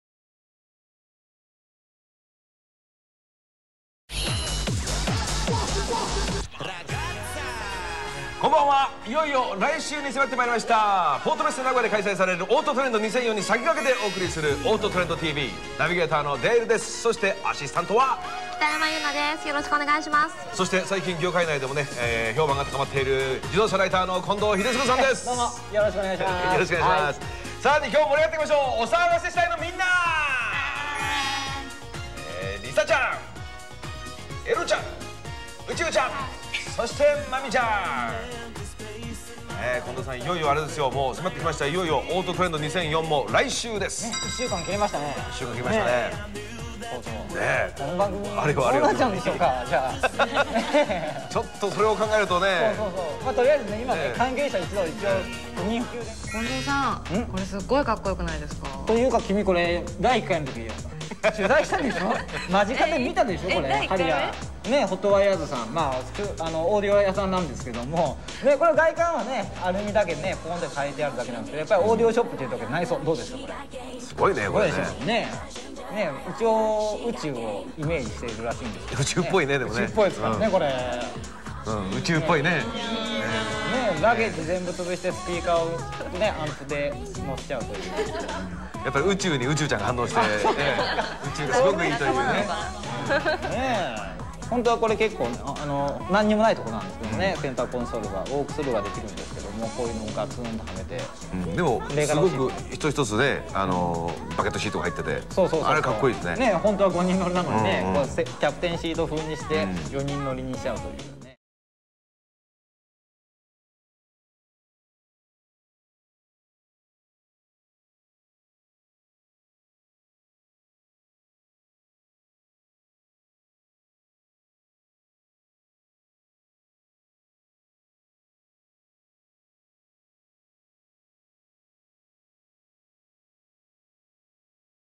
HOT WIRED製作 トヨタ10系ハリアー！！！ TOYOTA HARRIER AUDIO CUSTOM
ホットワイヤードにて2003年頃に製作されたハリアーです。
ビルシュタインの足に，当時ではまだめずらしかった２０インチホイールとピレリのタイヤ，レカロのSR3を４脚入れて，ハイスピードクルージングも乗ってる人はとても快適，安心。

メインのオーディオカスタムは，5.1CH.DVDシアター仕様。
ラゲッジをすべてウーハーとアンプで使用，ウーハーはメガホン効果でディープで迫力のある低音を再生してくれます。
スピーカー及びウーハーは，JL AUDIOで統一，アンプはオーディソンのシアター用アンプ１台ですべてのスピーカーをドライブしています。
ものすごく迫力のあるすばらしい出来映えのシアターカーでした。
すばらしいクルマでしたが，最後は盗まれて，禿鷹に食べられたガゼルのようになってしまいました。
バッテリーやスピーカーケーブルまで無くなっていたそうです。
オーディオの分まで保険に入っていて良かったね。

HOT WIRED　（ホットワイヤード）
〒455-0801 愛知県名古屋市港区小碓（おうす）1-424